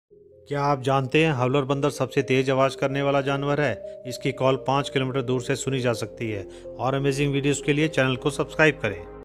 The loudest animal on earth : Howler Monkey
Discover the incredible vocal powers of the howler monkey, whose booming call is the loudest of any animal and can be heard from upto 5 km away.